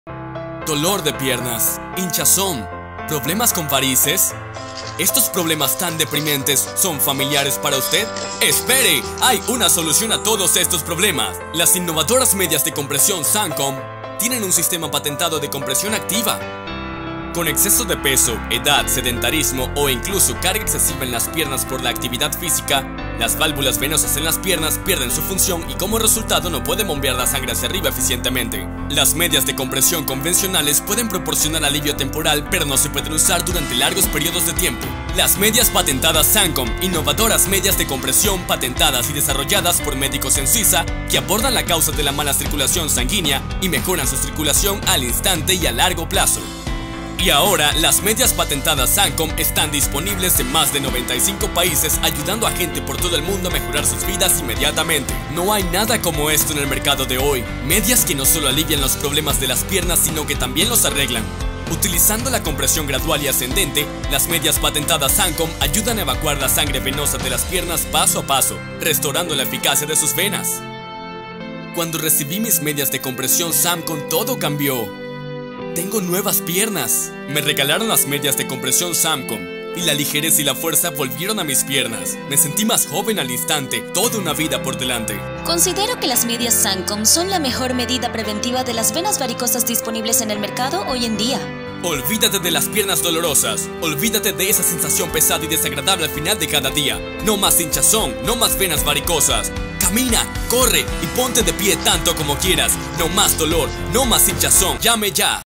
Medias Sankom - Teleshopping
Las medias Sankom son las primeras en el mundo que está clínicamente comprobado que mejoran la circulación y previenen el estancamiento de la sangre. o llámanos al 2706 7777.

Son fáciles de usar y mejoran la salud ayudando a la circulación sanguínea.

Tienen un mecanismo de comprensión gradual multinivel patentado que ayuda a evacuar efectivamente la sangre paso a paso. Proveen un alivio inmediato para la pesadez en las piernas con solo ponértelas.

Las medias Sankom están disponibles tanto para pantorrillas grandes como de tamaño regular. Tienen un área espaciosa para los pies y un talón acolchado para absorber los choques.

También tiene un arco especial y soporte en el tobillo para mantener el buen balance del cuerpo.

Las medias Sankom son muy fáciles de usar.

Es una invención patentada que está clínicamente comprobada.

Recibí las Medias Sankom en tu casa a un precio de oferta!